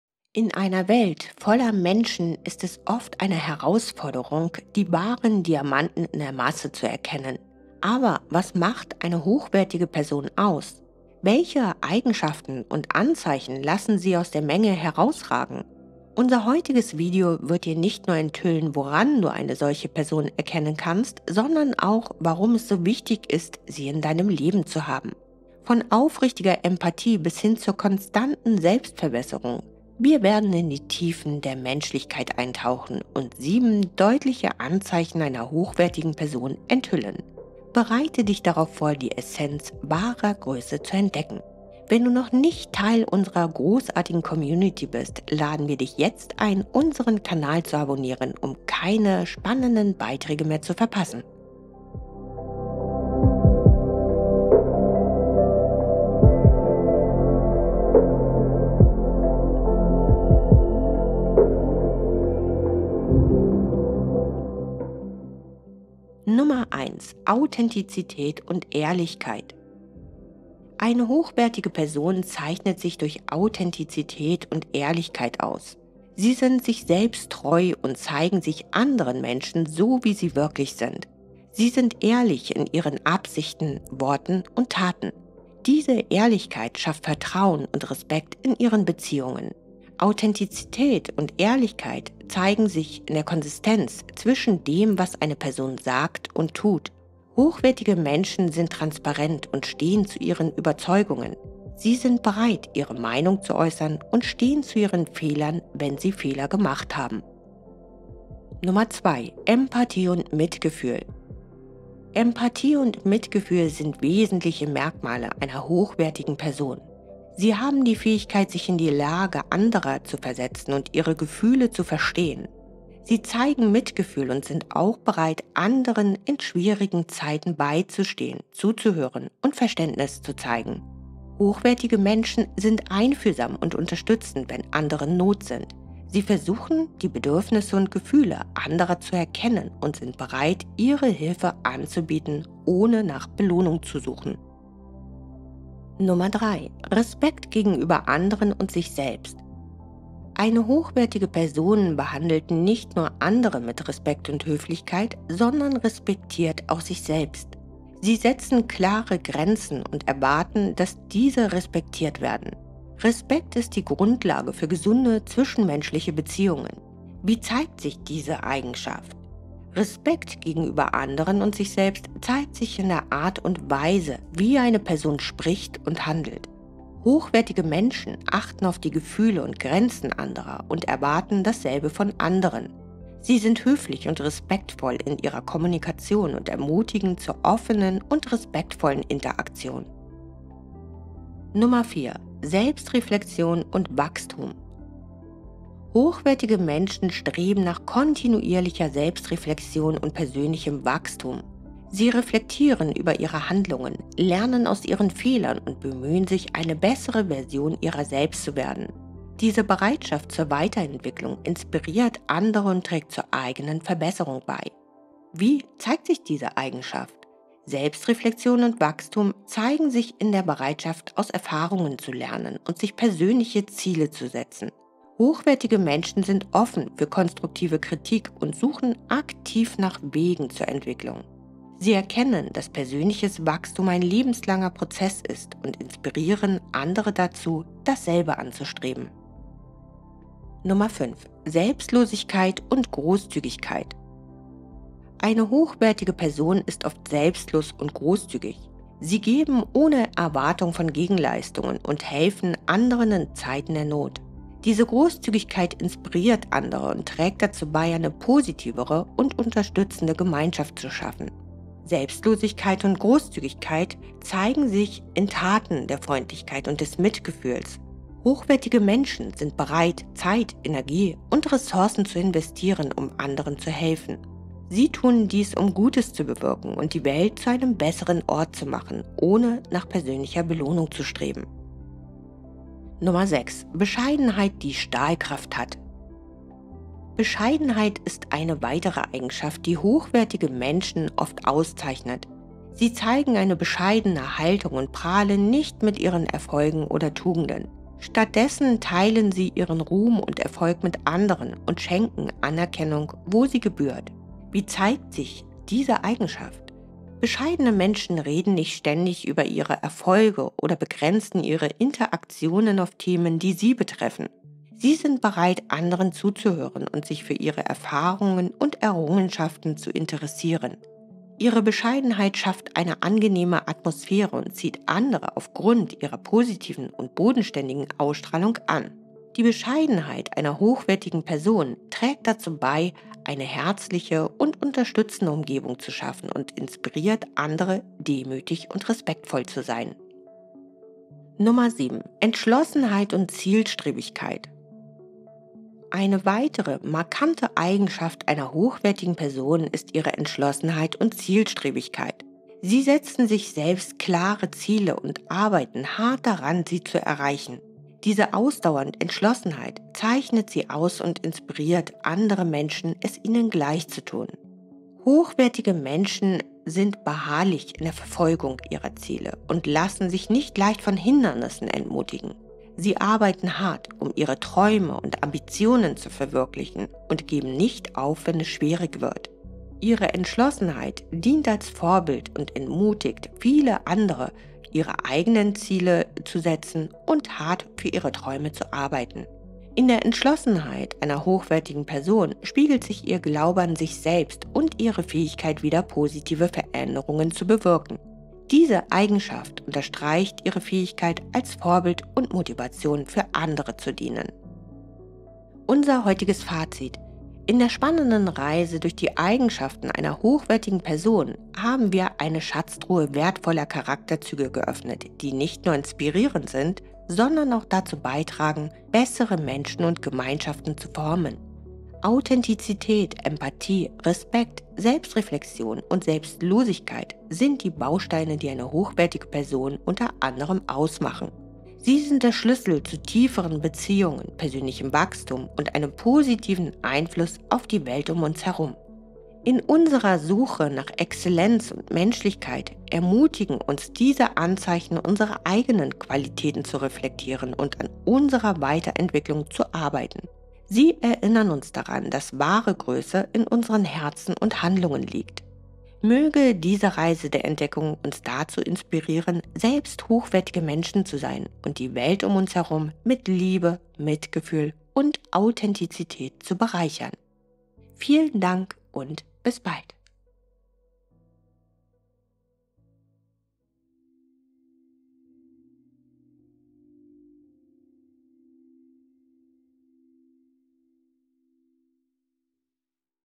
7 einzigartige Eigenschaften eines besonders wertvollen Menschen!
7 einzigartige Eigenschaften einer besonders wertvollen Menschen!

In einer Welt voller Menschen ist es oft eine Herausforderung, die wahren Diamanten in der Masse zu erkennen. Aber was macht eine hochwertige Person aus? Welche Eigenschaften und Anzeichen lassen sie aus der Menge herausragen? Unser heutiges Video wird dir nicht nur enthüllen, woran du eine solche Person erkennen kannst, sondern auch, warum es so wichtig ist, sie in deinem Leben zu haben. Von aufrichtiger Empathie bis hin zur konstanten Selbstverbesserung, wir werden in die Tiefen der Menschlichkeit eintauchen und 7 klare Anzeichen einer hochwertigen Person enthüllen. Bereite dich darauf vor, die Essenz wahrer Größe zu entdecken!